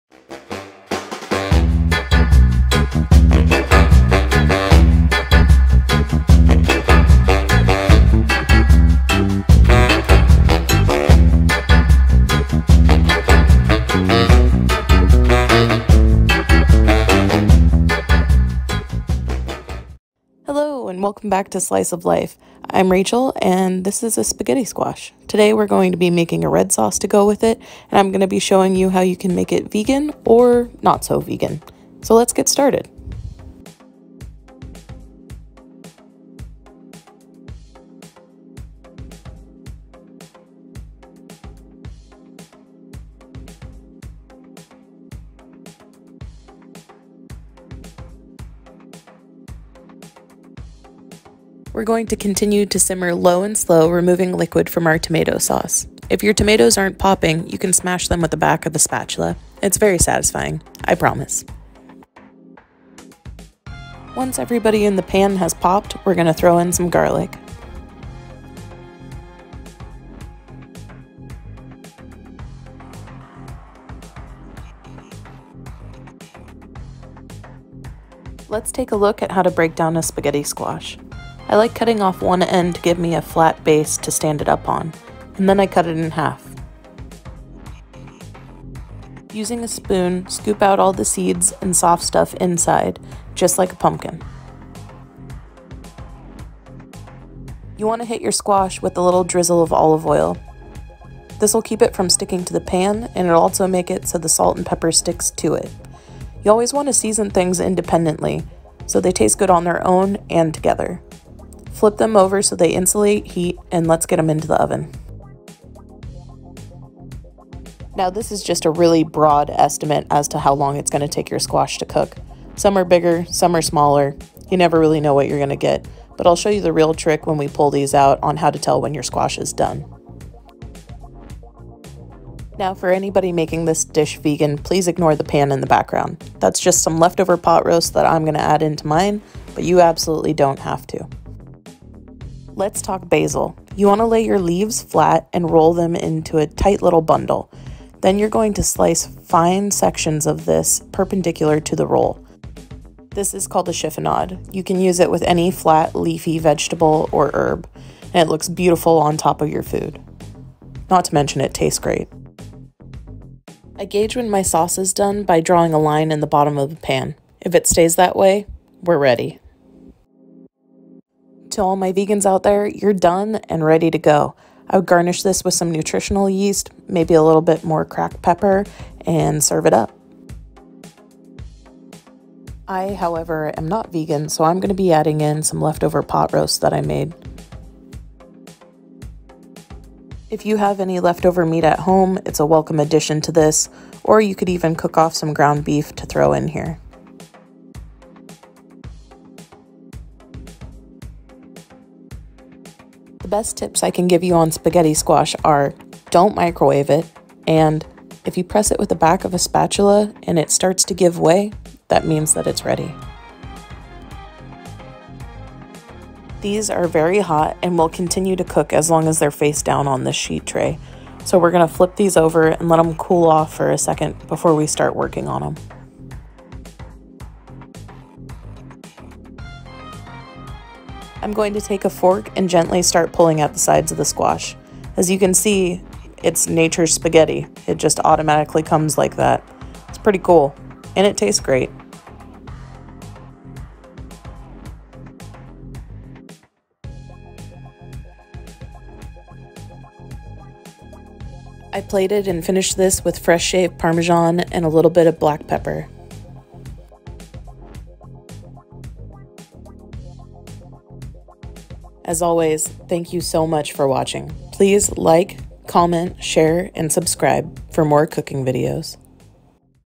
Red Sauce & Spaghetti Squash
Simple, healthy, and vegan if you want it to be!

I love listening to music while I’m cooking, It sets the moods for the foods. If you wanna make this in your own home, here’s some of my mixtapes: